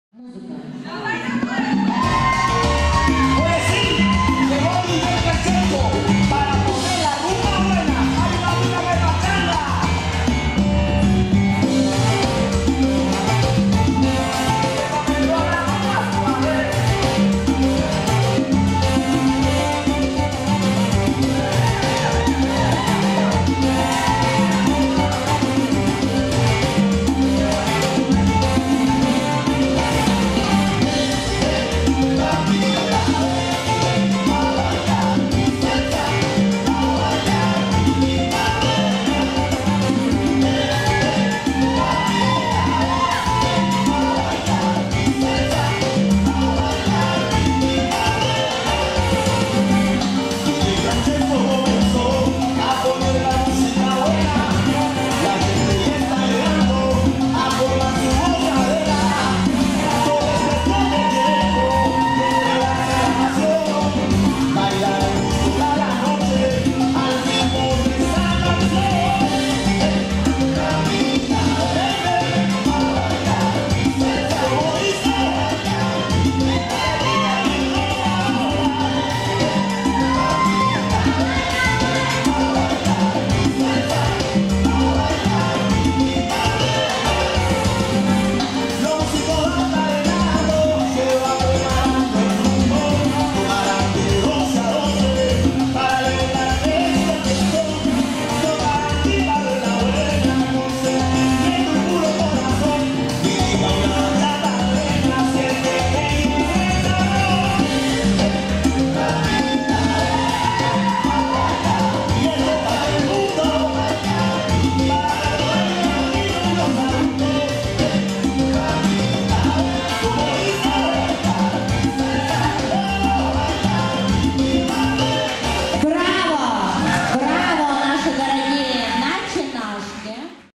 Copa de Oro 2018 - Libre Salsa Cubana - Principiantes 1/2 финала (1 заход) 1 - Ракурс 2
Copa de Oro 2018
г. Харьков, Танцевальный центр "Litvinoff Dance", 19 мая 2018 г.
Организаторы: Школа танца "ЛЕ Шкробтак"